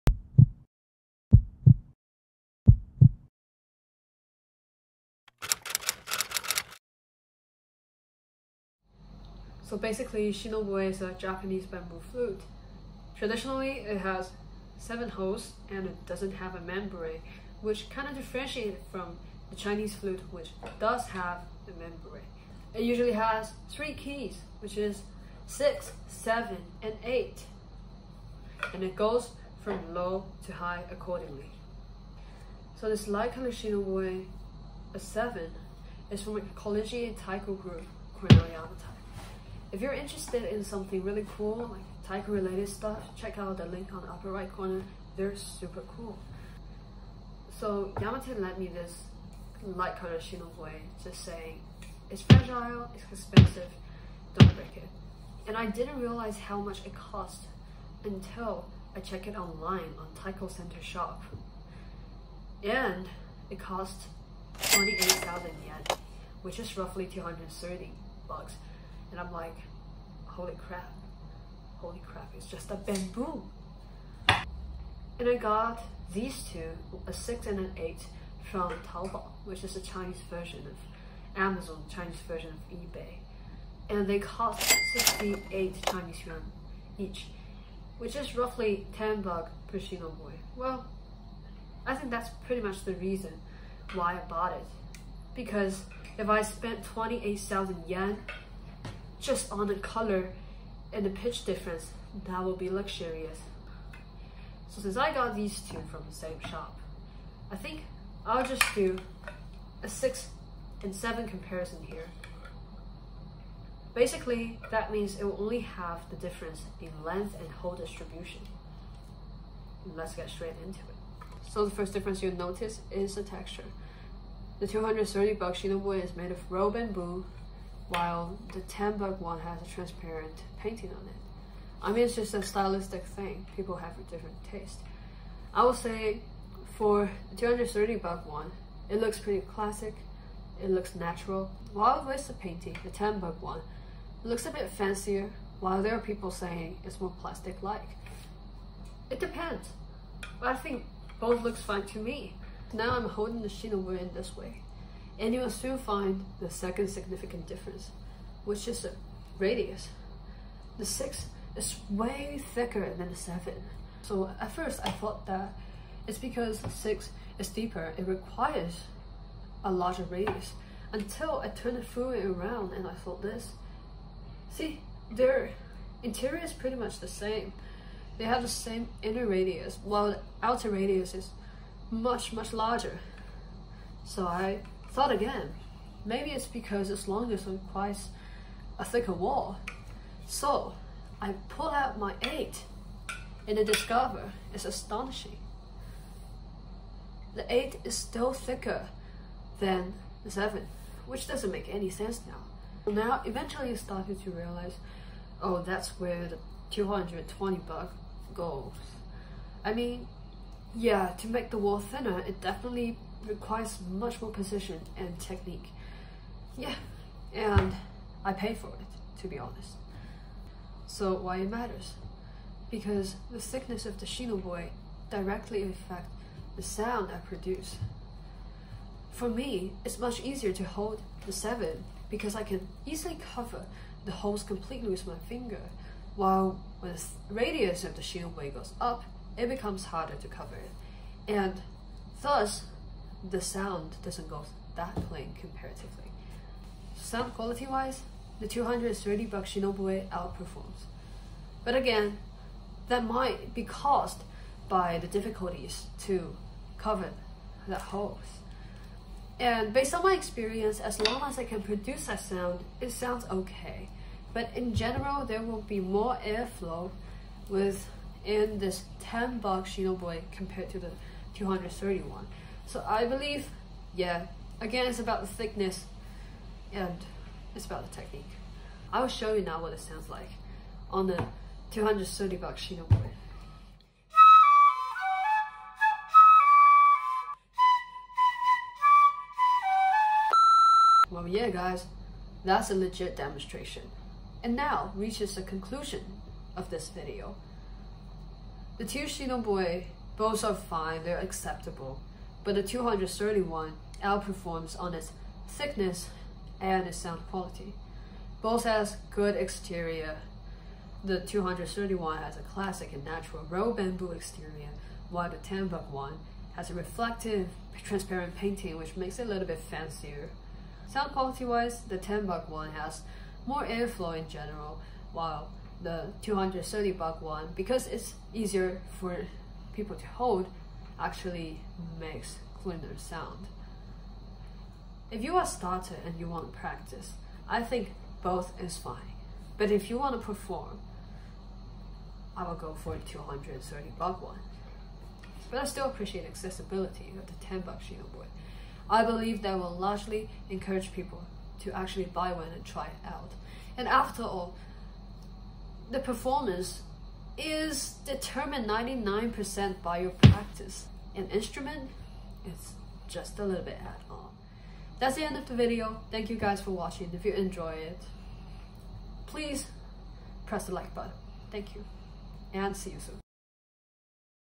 Shinobue Comparison: $10 VS $230?! What's the difference?
This video features a shinobue comparison: $10 VS $230.
Shinobue is a Japanese bamboo flute, and it's an important auxiliary in the taiko world.